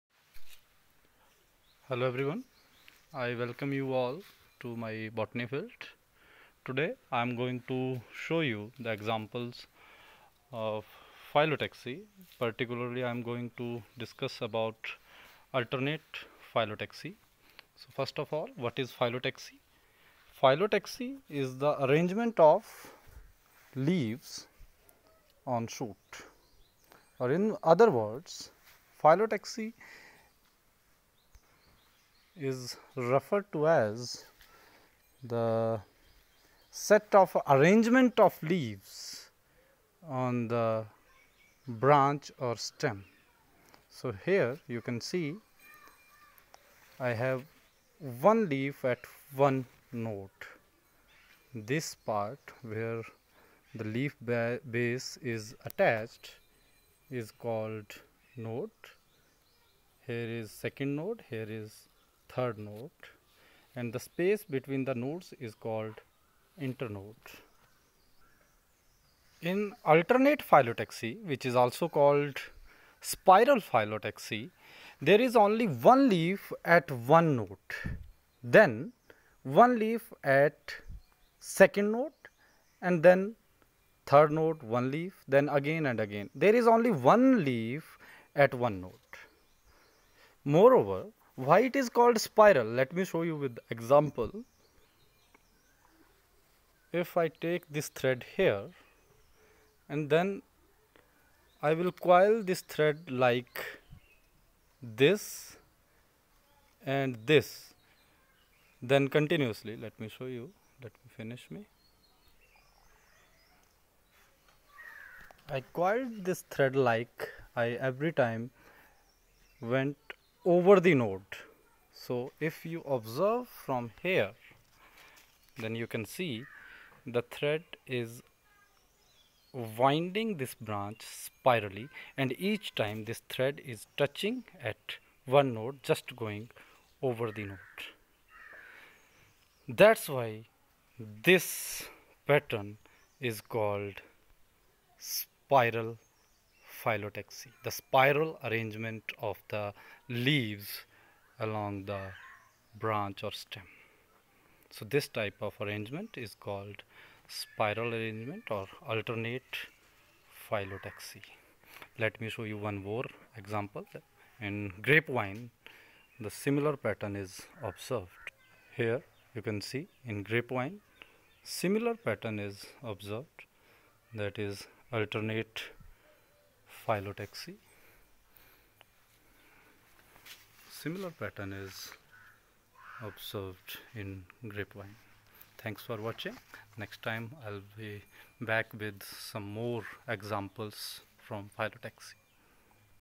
Alternate phyllotaxy
Phyllotaxy is arrangement of leaves on shoot
It can be
1. Alternate
2. Opposite
3. Whorled
Alternate phyllotaxy is explained here and rest will be explained in coming videos
Other examples of alternate phyllotaxy are Citrus, Chenopodium, Grevia, Morus and many more...